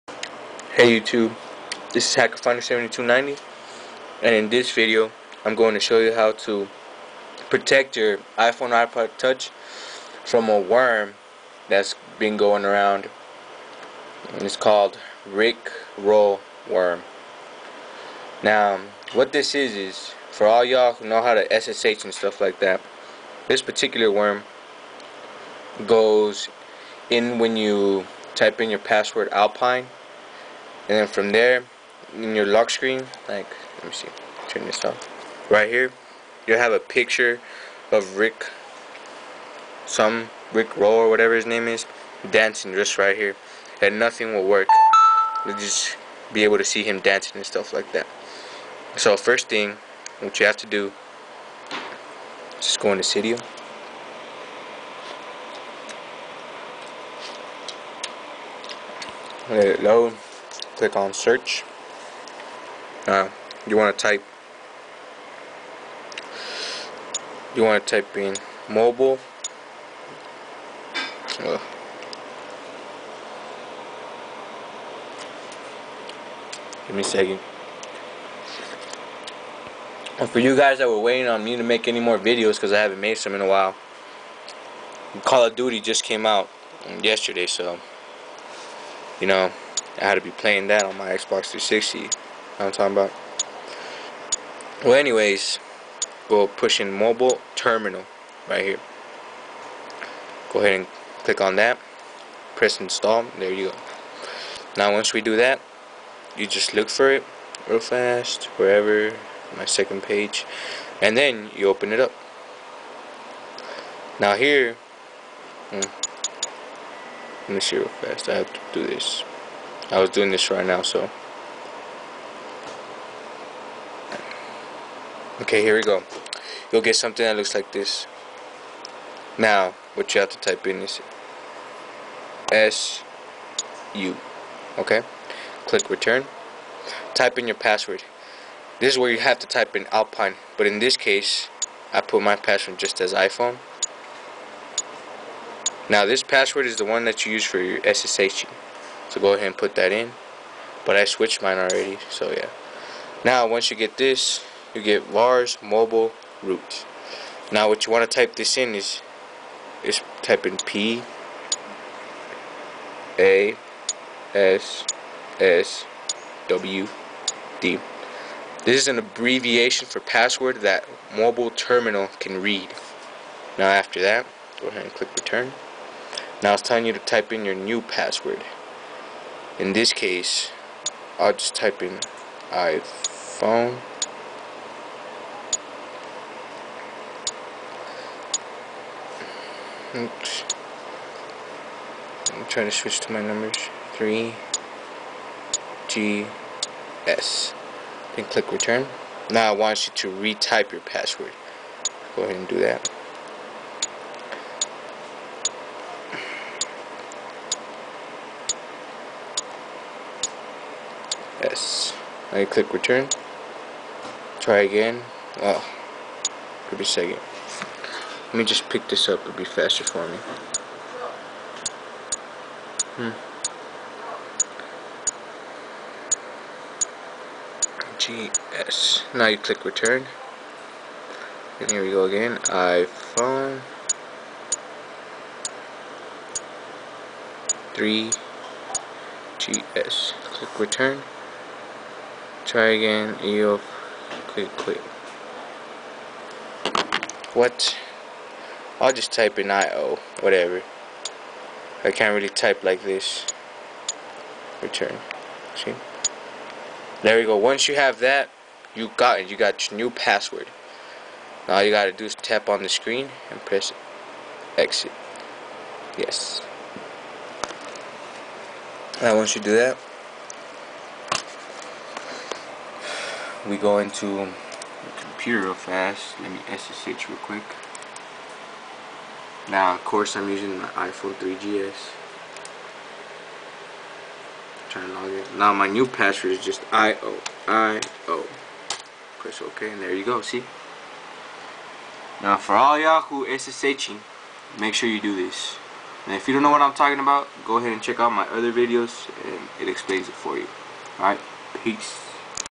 Protect your iphone from RckRoll Worm hack
This video is for all of you ssh ppl out there. You must do this in order to protect your iphone from being hacked...